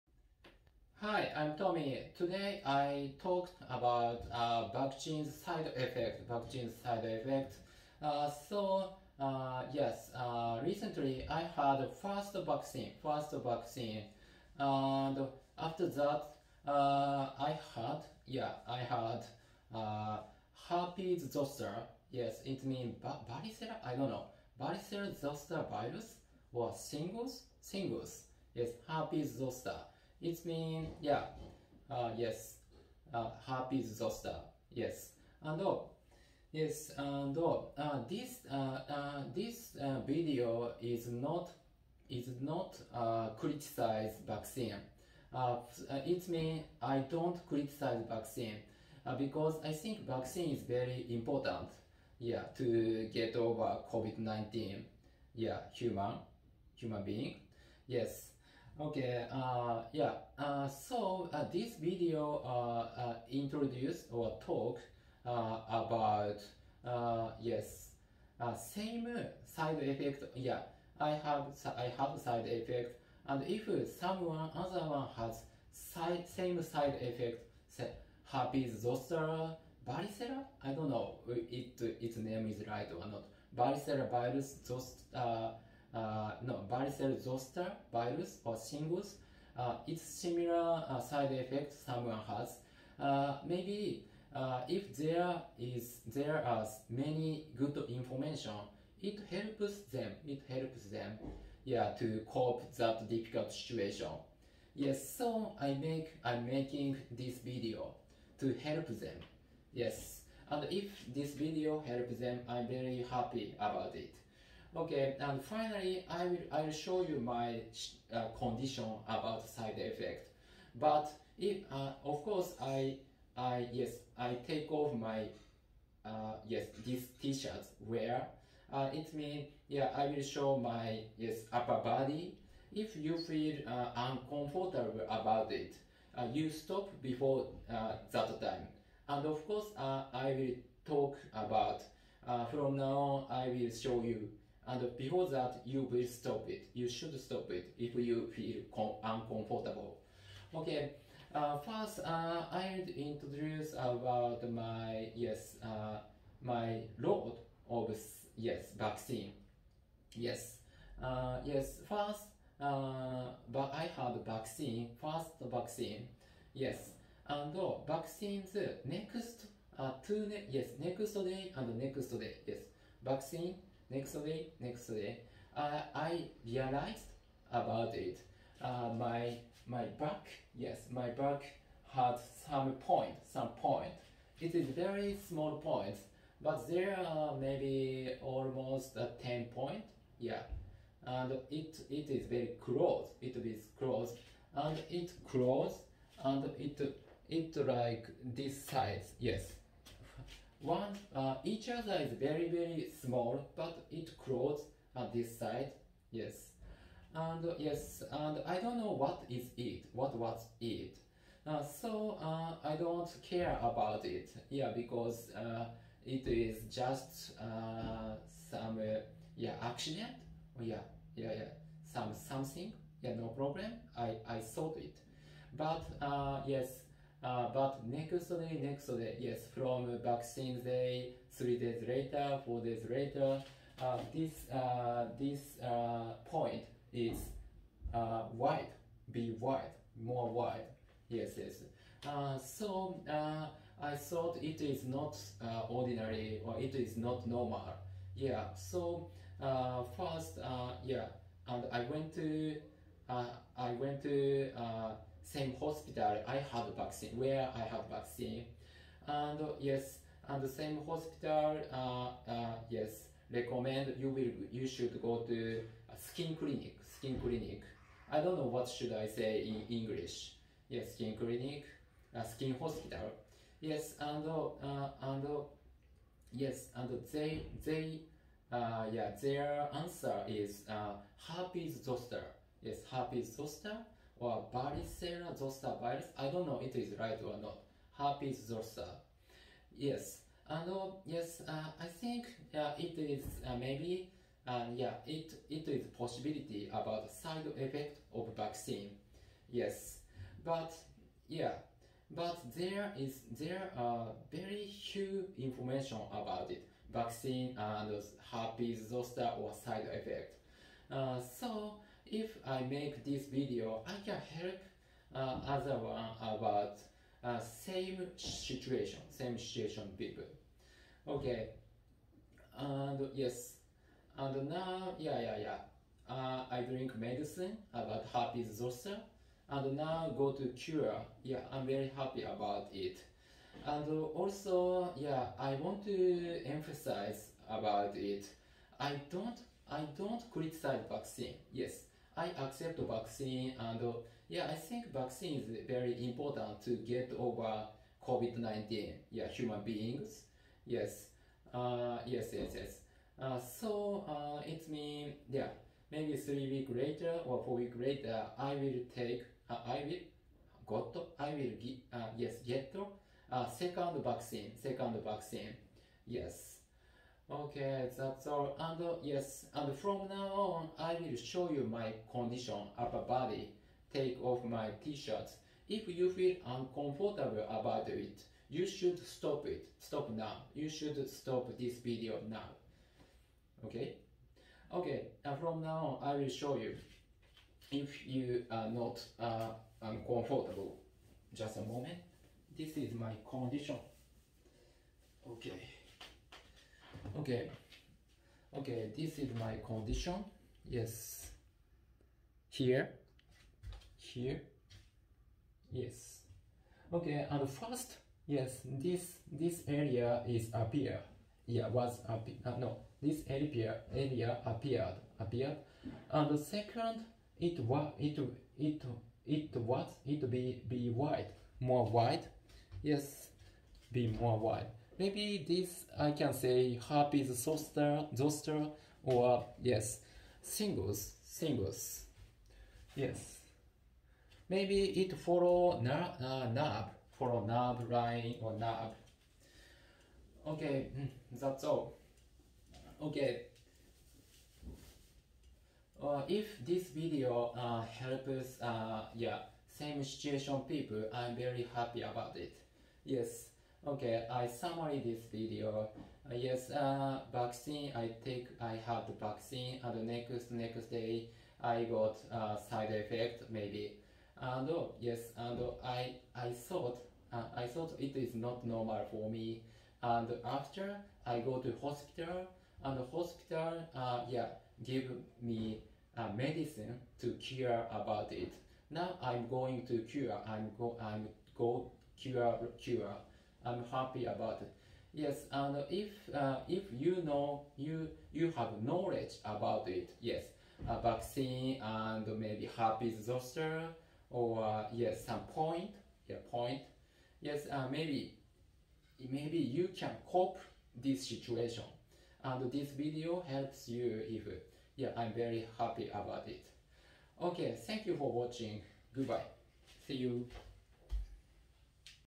I don't criticise vaccine.I got covid vaccine after that I had herpes zoster(point/rash/blister)
herpes zoster/varicella zoster virus/shingles